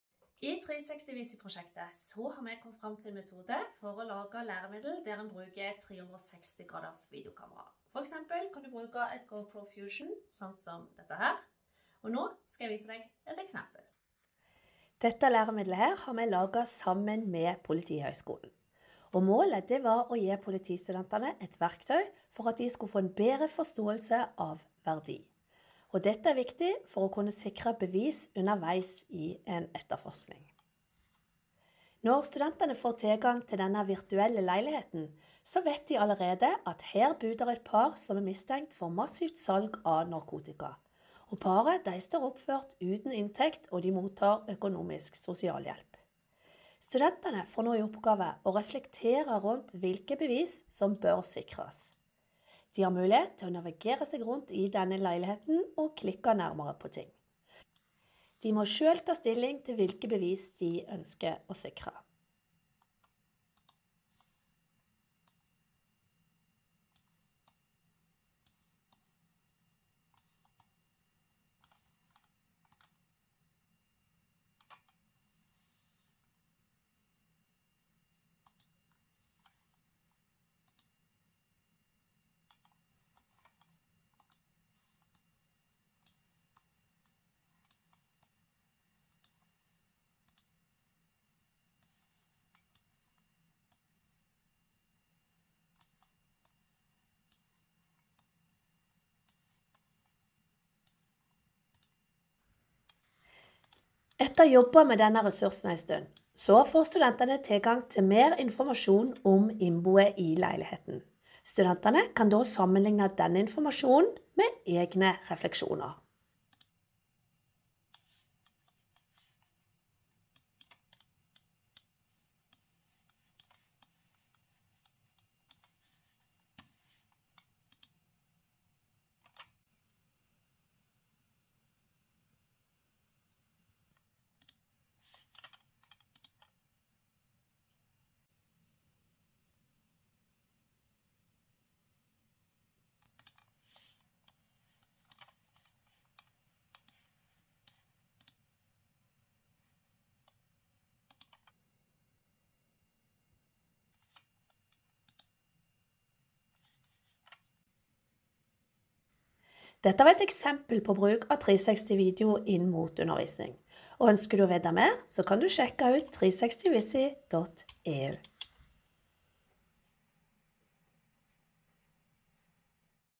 Eksempel på bruk av 360°video inn mot undervisning
Å kunne produsere interaktiv 360°video på en enkel måte inn mot undervisning, har vært utgangspunktet for EU-prosjektet 360ViSi
Mari Linn Larsen, videoprodusent ved NETTOP-UiS, viser her eksempel på en læringsressurs som baserer seg på denne type teknologi. Ressursen er utviklet i samarbeid med Screen Story og Politihøgskolen.